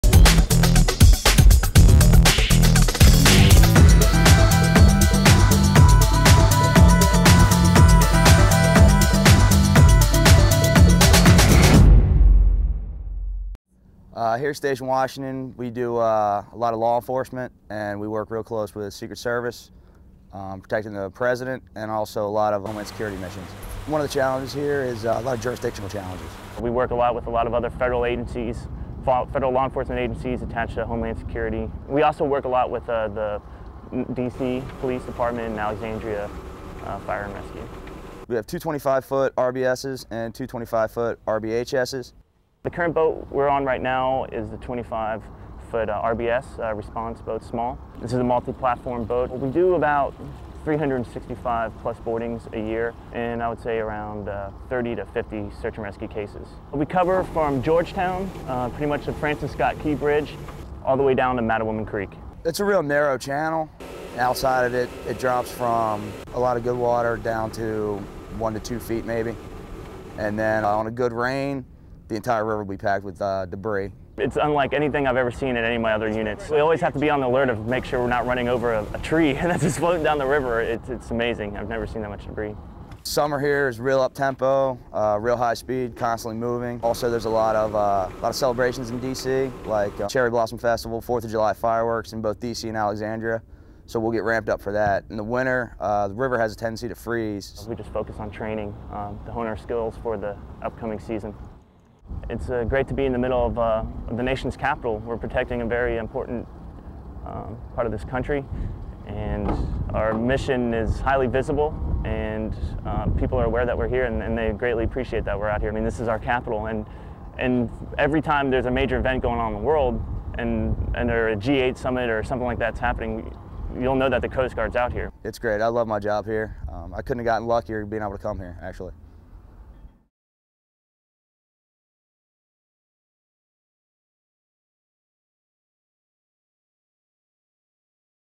Get to Know: Station Washington D.C.
Originally staffed with reserve and temporary duty boat crews, today there is a permanent unit.